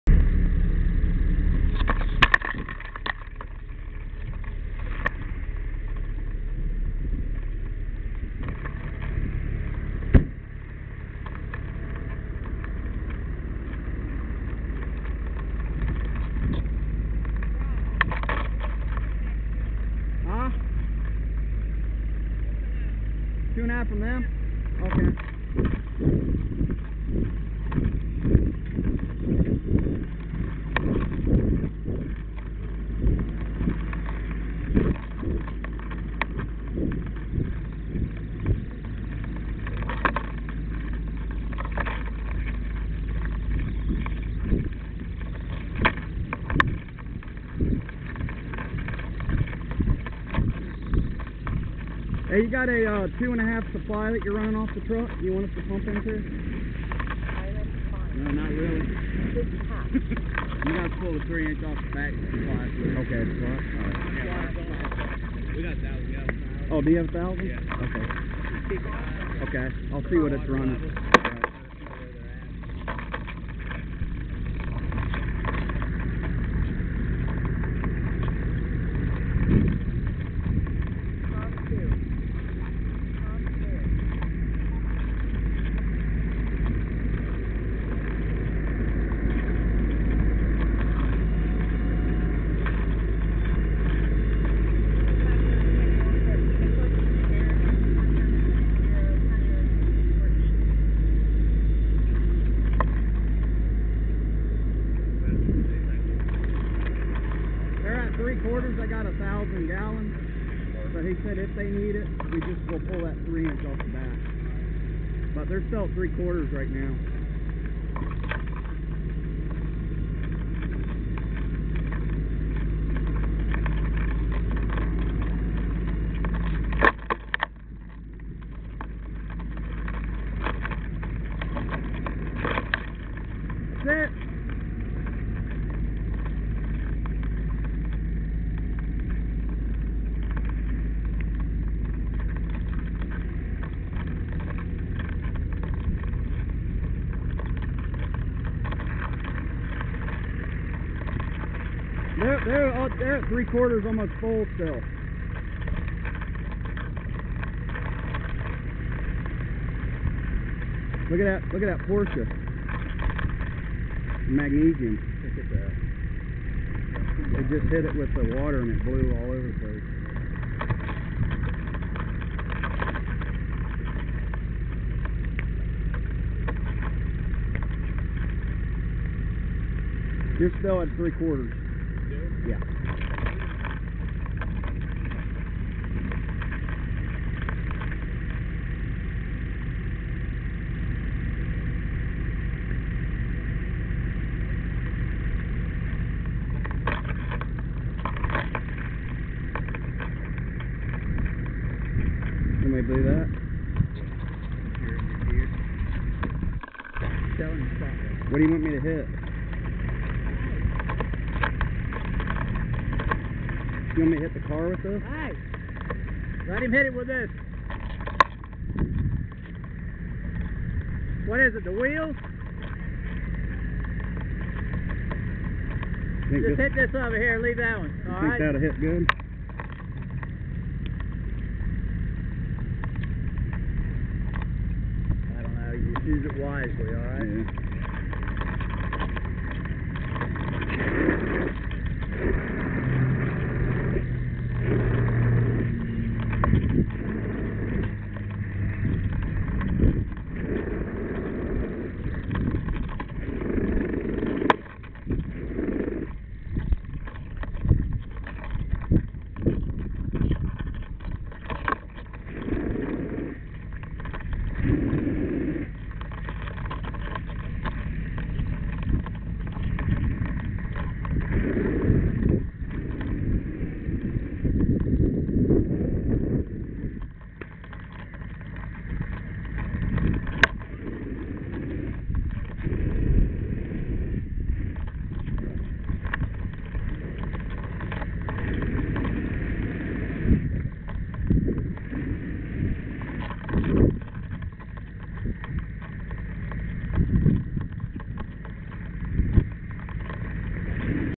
APFD Cars/ Brush Fire
Mutal Aid brush fire with cars involved.